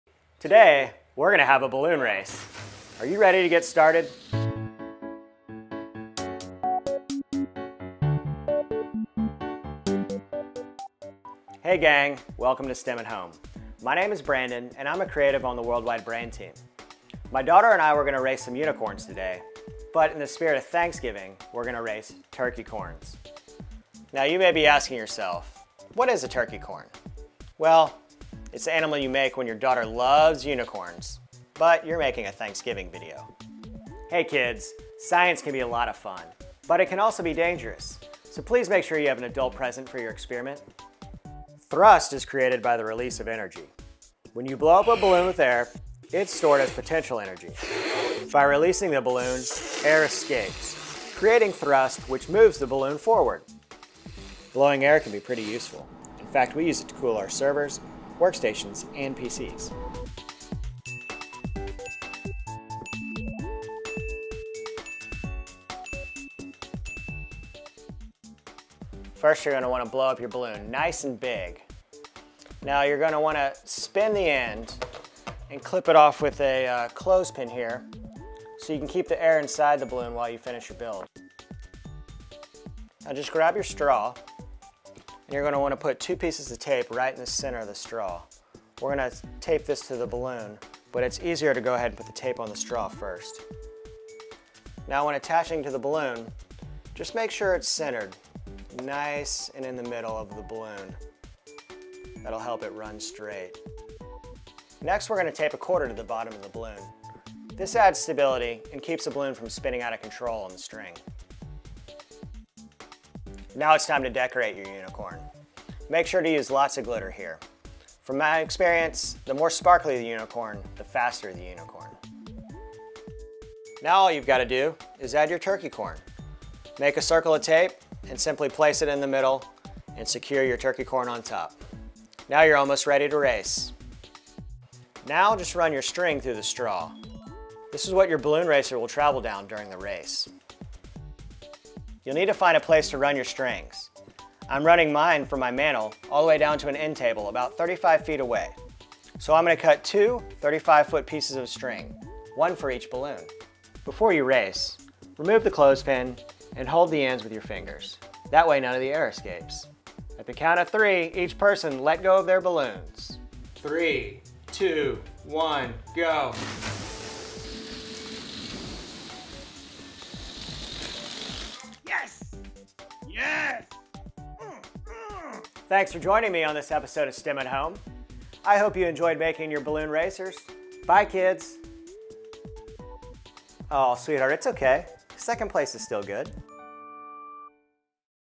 STEM at Home Episode #11: Building turkeycorn balloon racers
What do turkeys, unicorns and thrust have in common? Nothing until now. Join Lenovo Senior Creative, Brandon Willingham, as he builds turkeycorn balloon racers on this STEMatHome.